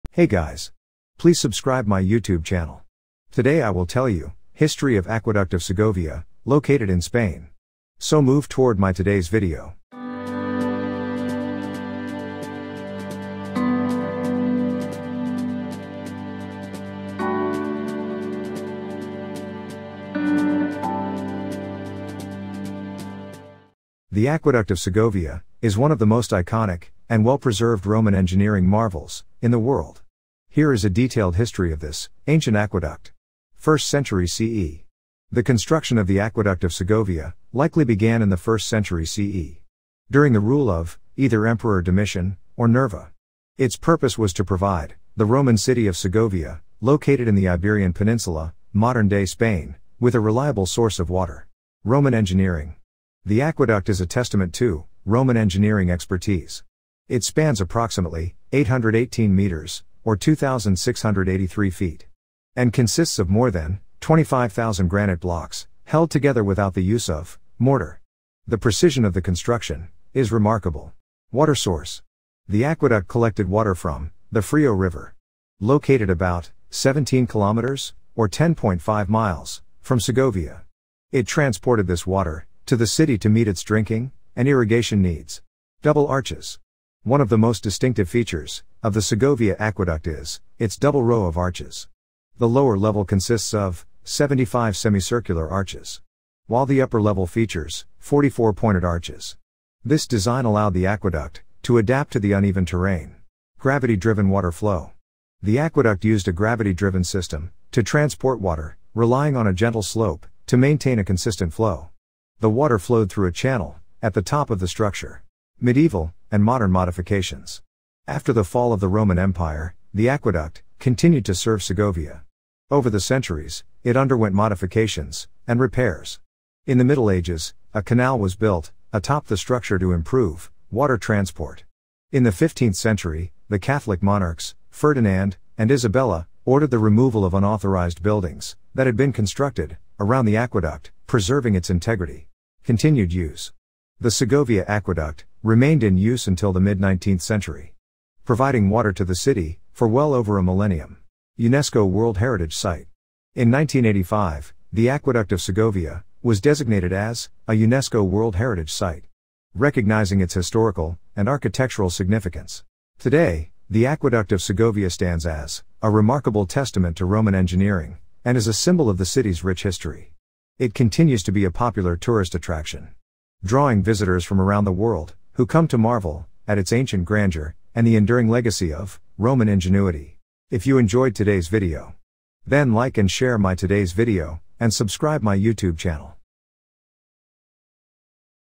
Aqueduct of Segovia: A Roman Masterpiece Through Time
Journey back in time with us as we uncover the rich history and architectural brilliance of the Aqueduct of Segovia, a true marvel of Roman engineering. Discover the story of its construction, purpose, and the enduring legacy it has left on the landscape of Segovia, Spain.

🏛️ Roman Ingenuity: Explore how the Romans, in the 1st century AD, designed and built this colossal aqueduct without mortar. Over 25,000 granite blocks pieced together to create this iconic structure.

🌊 Water Supply: Learn about the aqueduct's vital role in supplying fresh water to the city of Segovia, ensuring the well-being and prosperity of its inhabitants.

🏞️ Architectural Wonders: Marvel at the iconic double row of arches, both semi-circular and pointed, and understand how this engineering marvel adapted to the region's rugged terrain.

🔧 Maintenance and Preservation: Discover how the aqueduct was continually maintained and even adapted throughout the centuries, showcasing its endurance and adaptability.

🌍 UNESCO Heritage: Recognize the global importance of the Aqueduct of Segovia, designated as a UNESCO World Heritage Site, and the indelible mark it has left on the cultural heritage of Spain.

Join us as we delve into the history of the Aqueduct of Segovia, a testament to human innovation and an enduring symbol of the ancient Roman world.